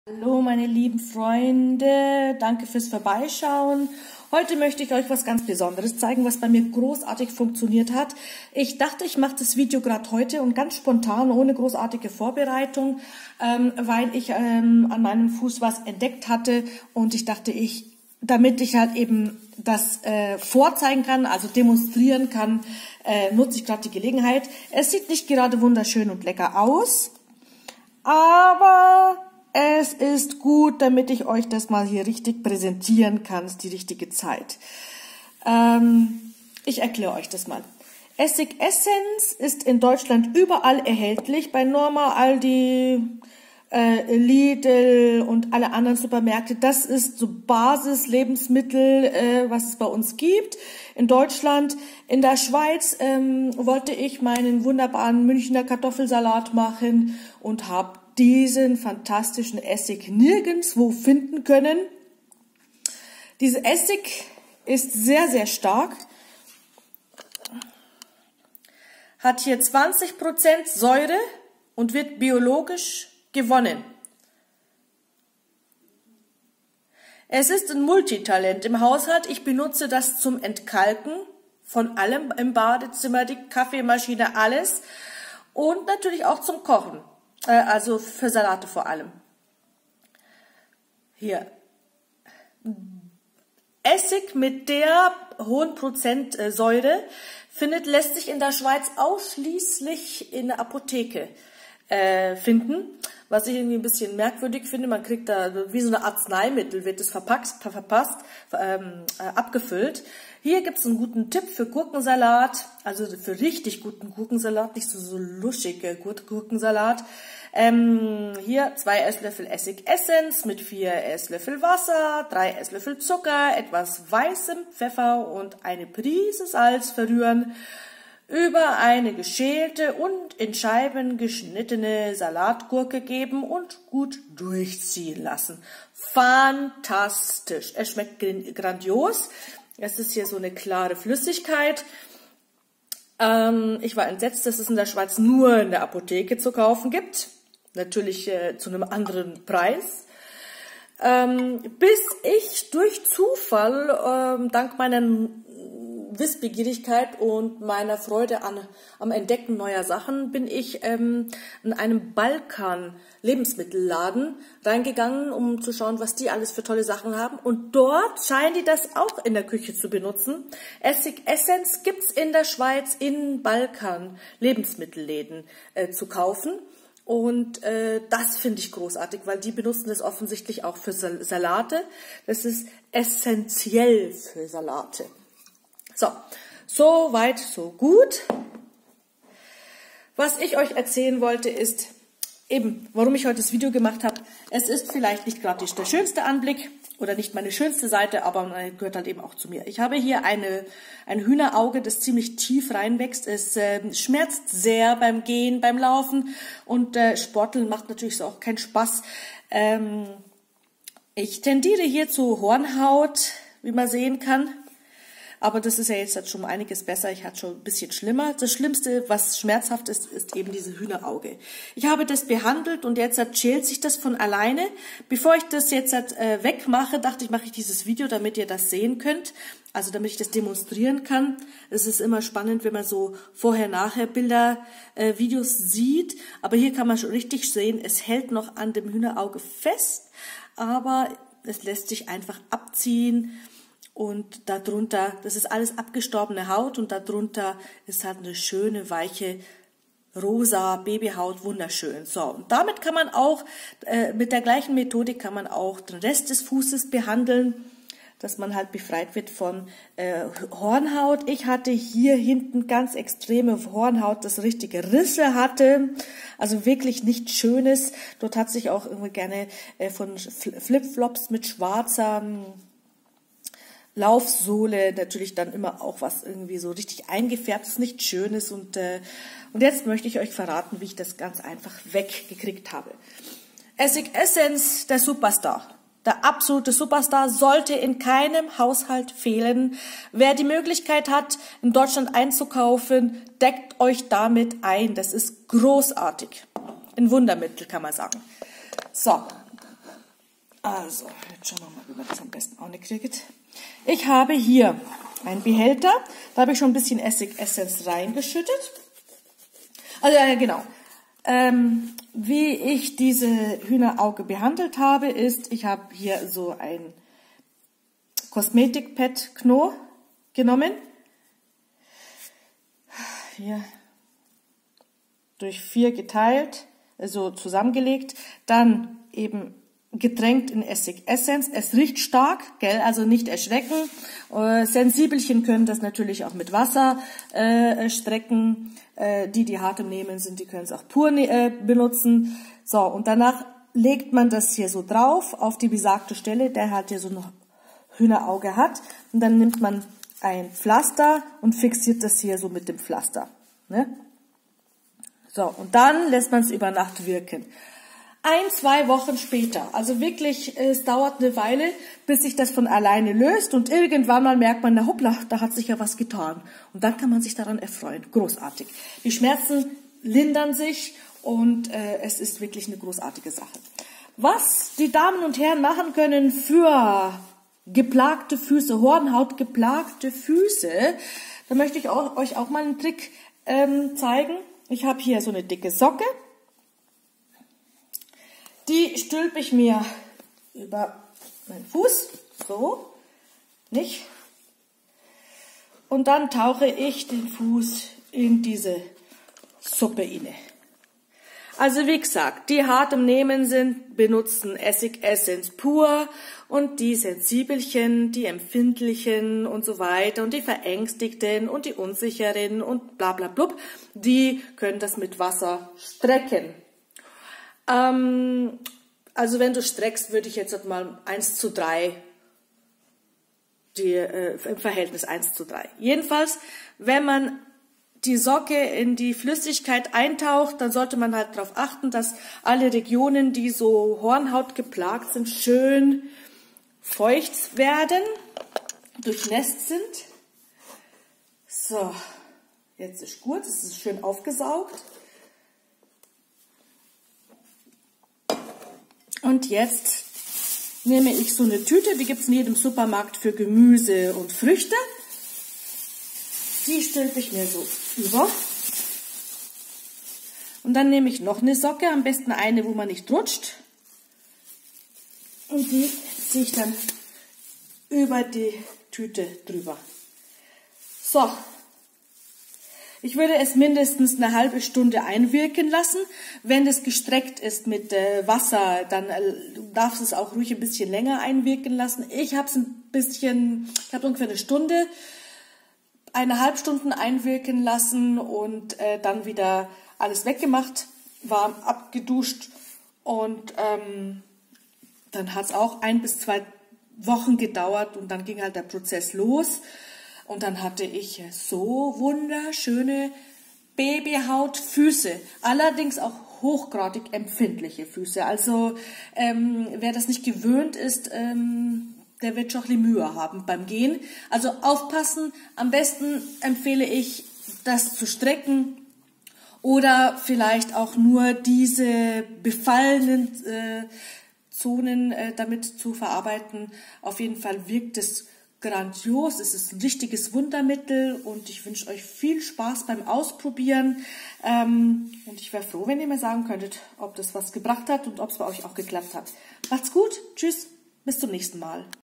Hühneraugen und Hornhaut Killer Essig Essenz
funktioniert radikal, Essig Essenz von Carl Kühne darf in keinem Haushalt fehlen.
das Ergebnis wird begeistern und überzeugen.
Versuch es selbst und sag es weiter.